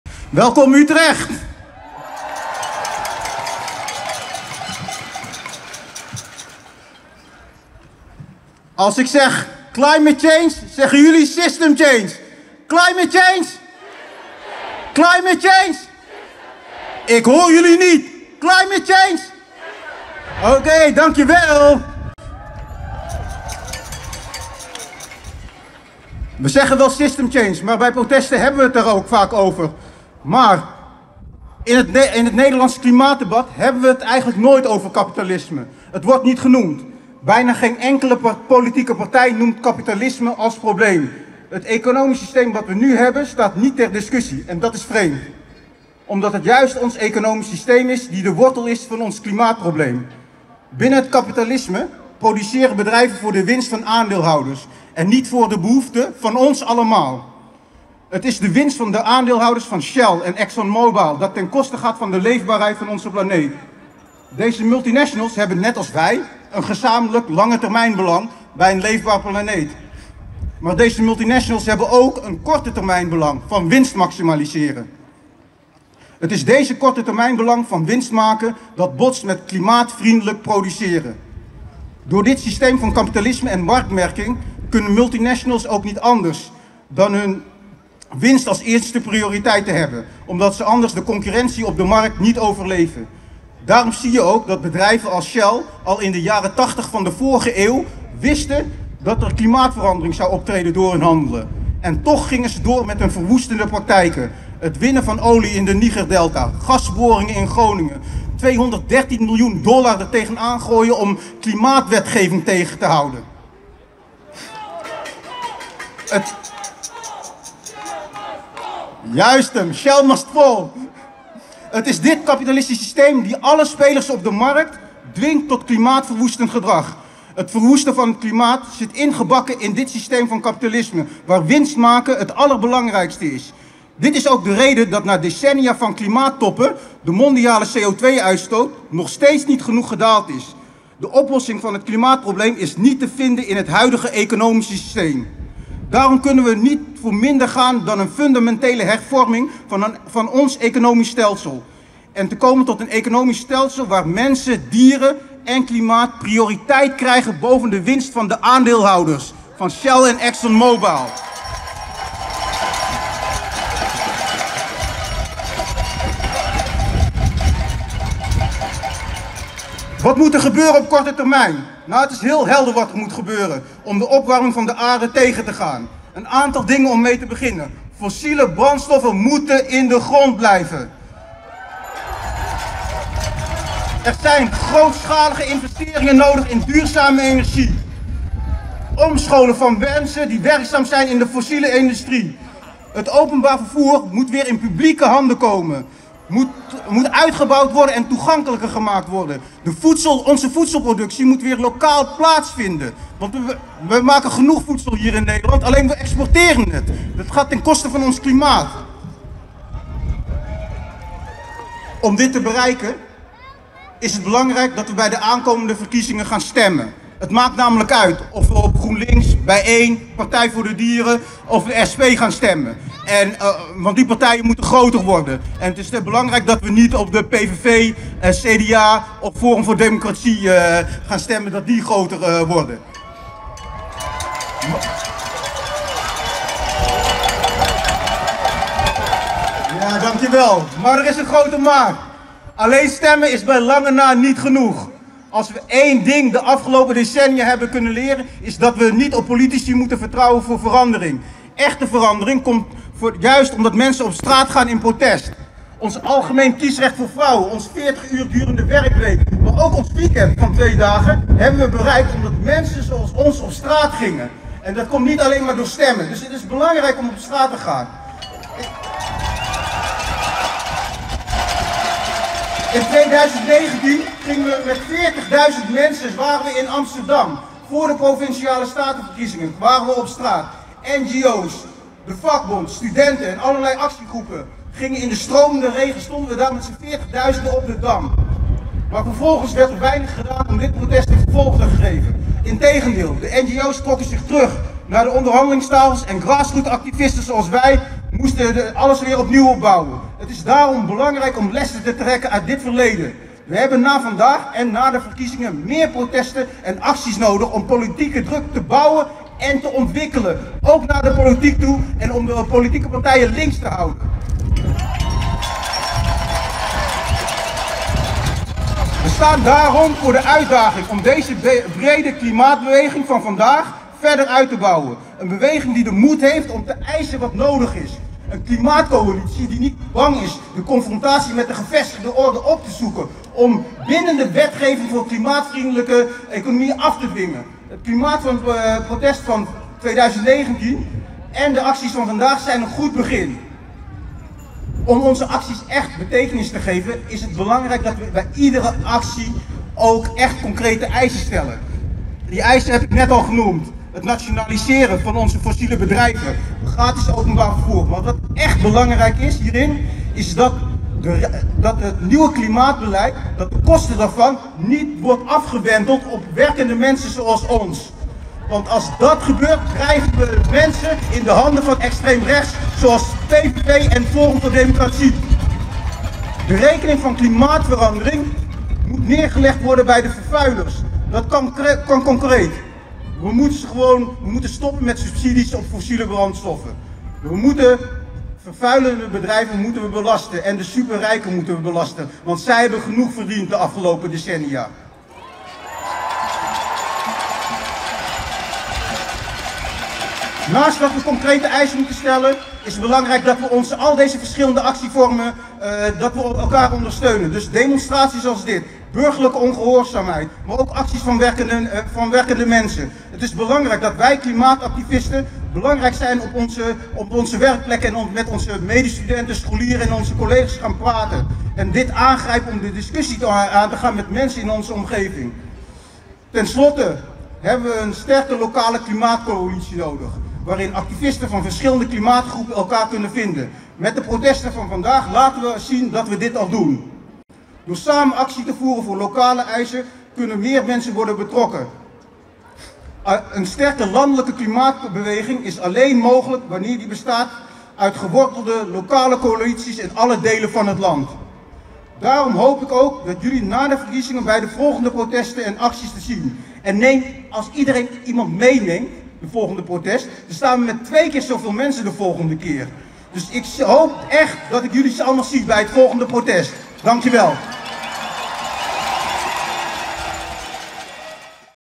Toespraak: System change not climate change!
De toespraak van Milko van de Internationale Socialisten gisteren tijdens het klimaat alarm in Utrecht. Juist het kapitalisme is de wortel van het klimaat probleem, het is de winst van bedrijven als Shell en Exxonmobile dat ten koste gaat van de leefbaarheid van onze planeet.
Wat kan je doen? Stem links, maar alleen stemmen is niet genoeg. Organiseer je en help mee de beweging op te bouwen! (opname: Oscar Brak)